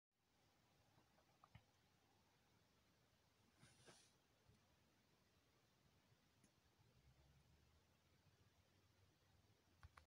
Do you have this DVD #12
No! But I do have Toy Story 4 on DVD of Chirstmas of 2019, but I might get the 1st one at JB Hi-Fi or Sanity soon?